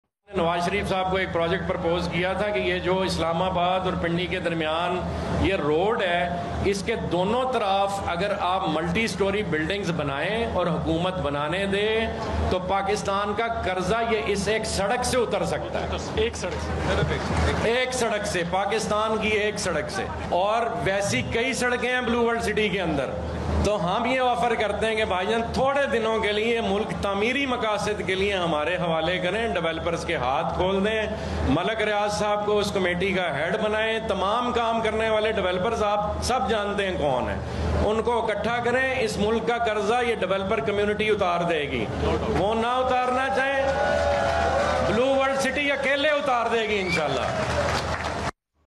Blue World City Islamabad Balloting | Saad Nazir Mega Announcements
In this video, we bring you the latest updates on the highly-anticipated Blue World City Islamabad balloting and the exciting mega announcements made by Saad Nazir.

As one of the most sought-after real estate projects in Islamabad, Blue World City has been making waves with its innovative approach and stunning infrastructure. During the balloting, lucky winners received plot numbers as per their preferences, securing their investment in this prime location.

Saad Nazir, the CEO of Blue World City, also made some massive announcements that left everyone buzzing with excitement. From special discounts to new features, there's something for everyone to look forward to!